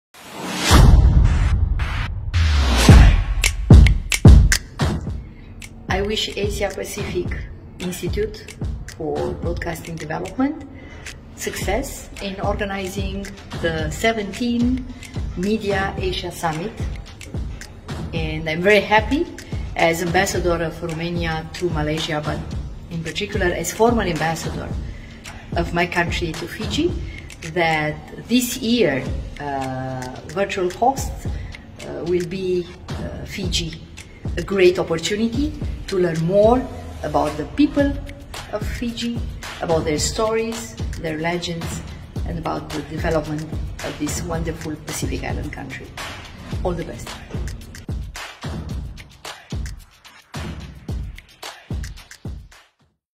AMS 2022, H.E Nineta Bărbulescu, Romanian Embassy in Kuala Lumpur, Malaysia
H.E Nineta Bărbulescu, the ambassador of Romanian Embassy in Kuala Lumpur, Malaysia send in her wishes for our upcoming 17th Asia Media Summit 2022.

In just 8 days, the 17th Asia Media Summit will be coming on live to your eyes! Register now to secure your spot!